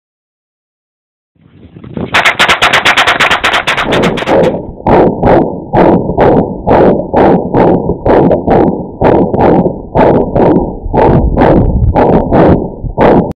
Dual Smith & Wesson 22A Pistols
Dual pistols, S&W 22A shooting Federal Bulk pack .22 caliber ammunition. The 22A is a solid plinking pistol and comes highly recommended for beginners.